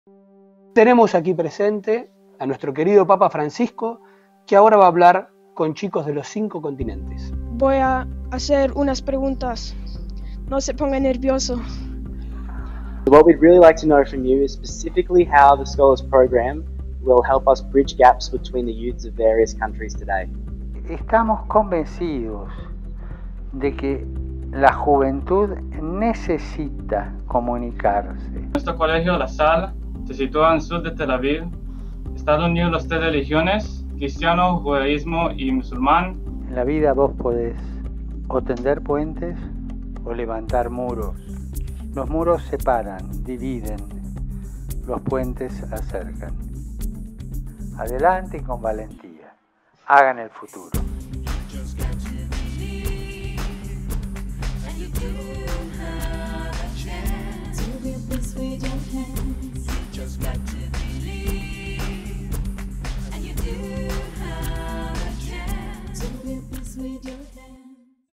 Scholas Hangout Trailer (Eng)
This Thursday 2/5 at 4pm (CET), the Pope will reach out to children with disabilities and special needs in a live conversation from the Vatican. Everyone is welcome to follow this unique encounter, made possible by technology, at Scholas G+ page.